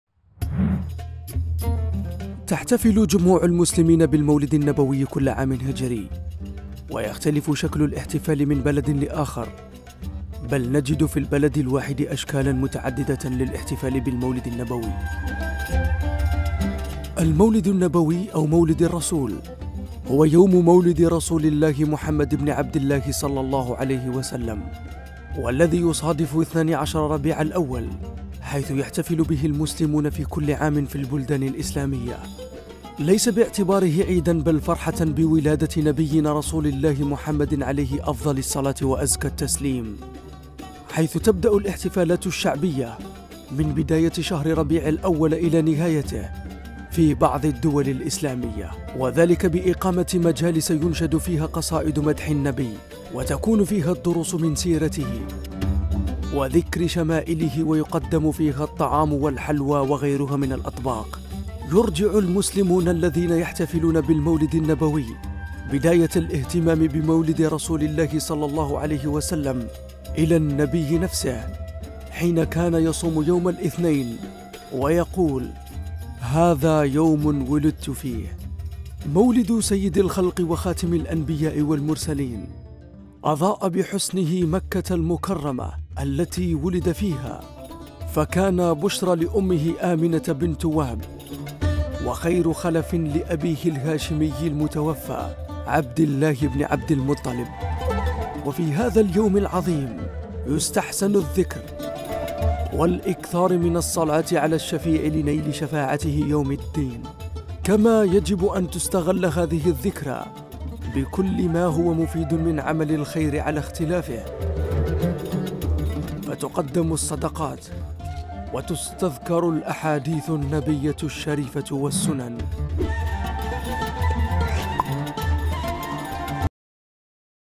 هكذا يحتفل المسلمون بمولد خاتم الأنبياء والرسل "محمد عليه الصلاة والسلام"
DJplus ... تقدم لكم قصص انسانية مختلفة ومتنوعة